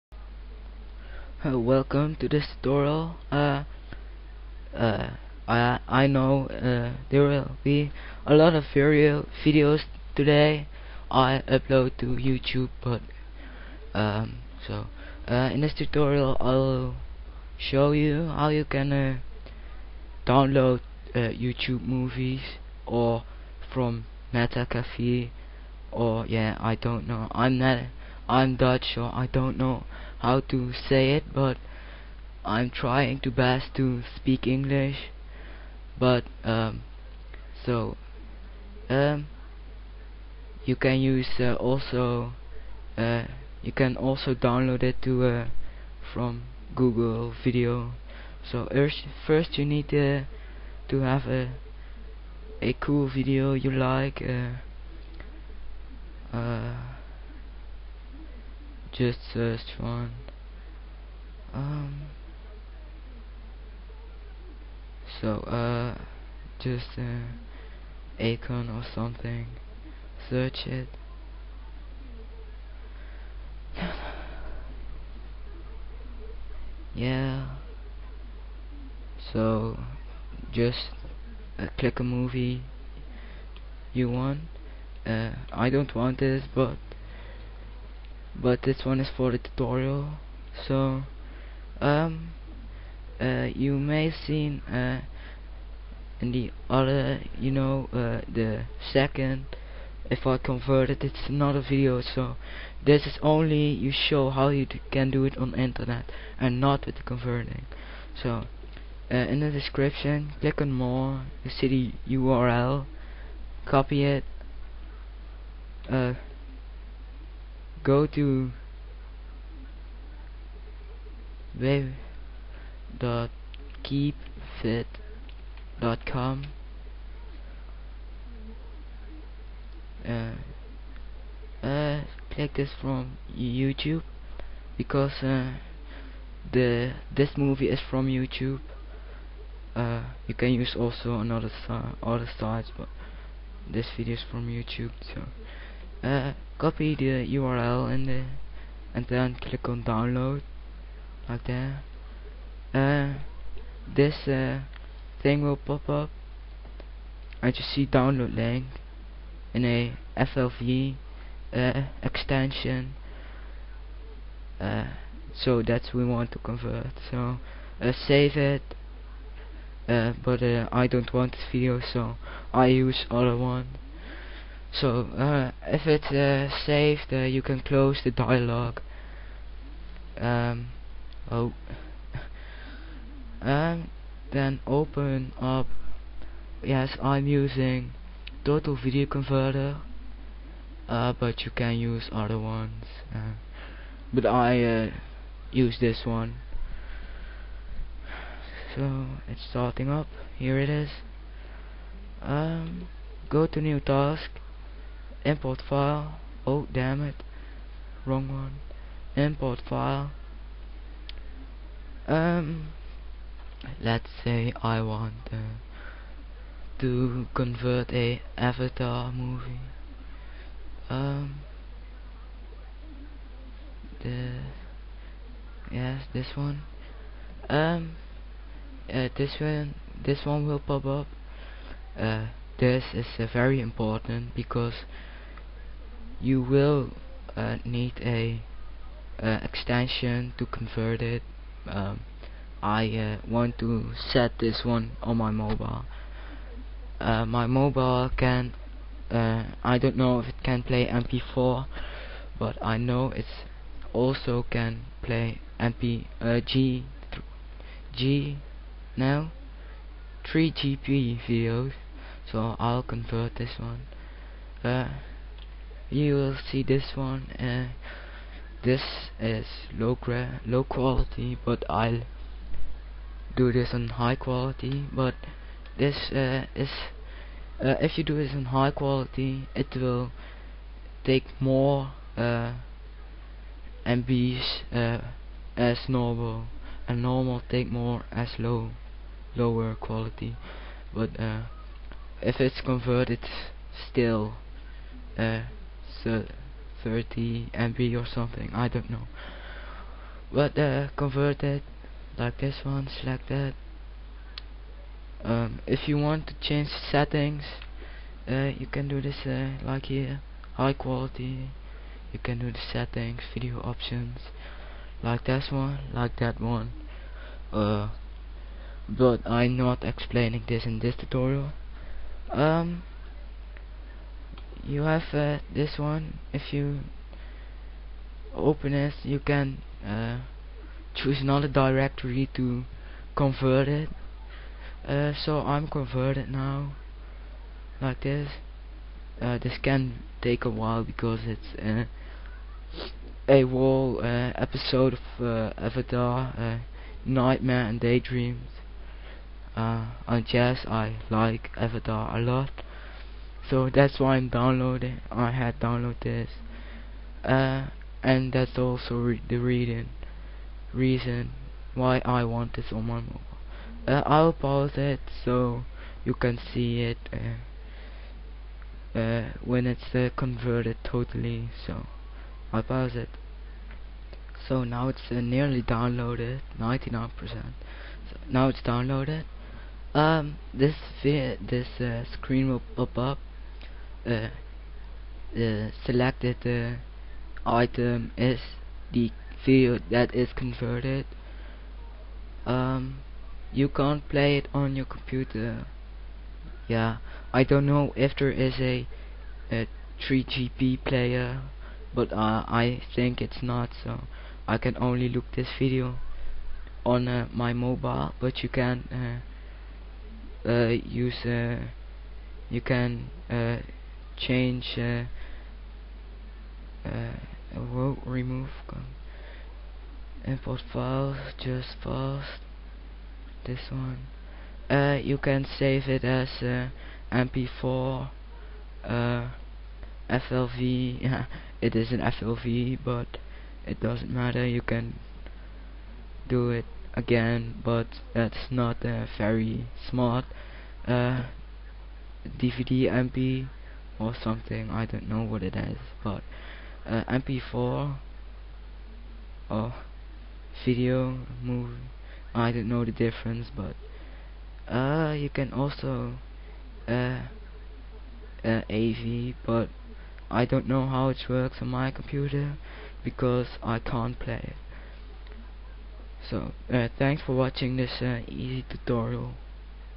How To Download A Youtube Movie And Convert it!
This tutorial will show you how to download Youtube movie's or from other site's And Convert it. Please Note:
Keepvid is Broken!